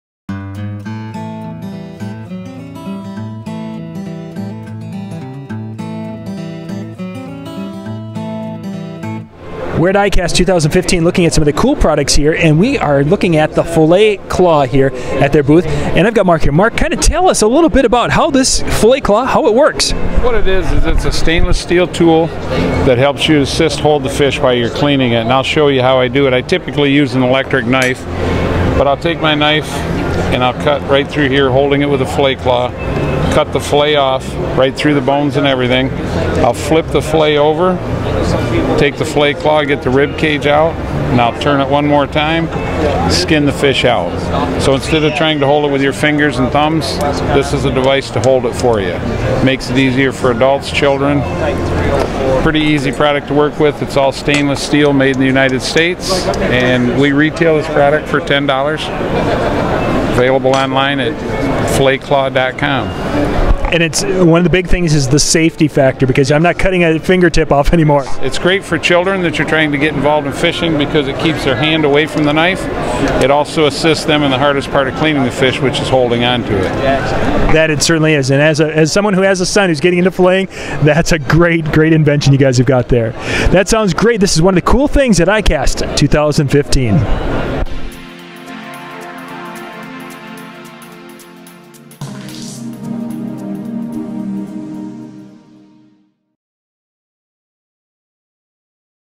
Filet Claw Fish Holding System | Weekend Sportsman
A retired handyman and weekend fisherman from Jackson, Michigan became discouraged with the effort it took to fillet his BIG CATCH!  He was unable to hold the fish firm against the slimy surface and his finger would slip off making it extremely difficult to fillet. To  prevent continuous stress on his hand and fingers he shopped every local sporting goods store in town, only NOT to find a single tool that fit his need.